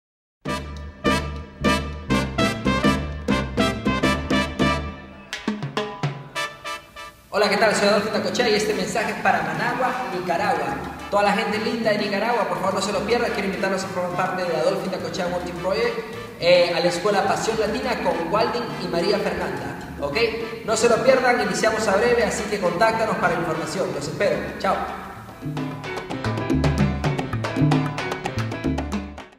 Adolfo Indacochea te invita al Team Managua
Únete a Adolfo Indacochea World Team Project Managua
Team Trainers: Waldin Joseph Ramos sanchez y Maria Fernanda itza.
Academia : Pasión Latina Dance Project Nicaragua.
Dirección : Fandango "la casita de la salsa"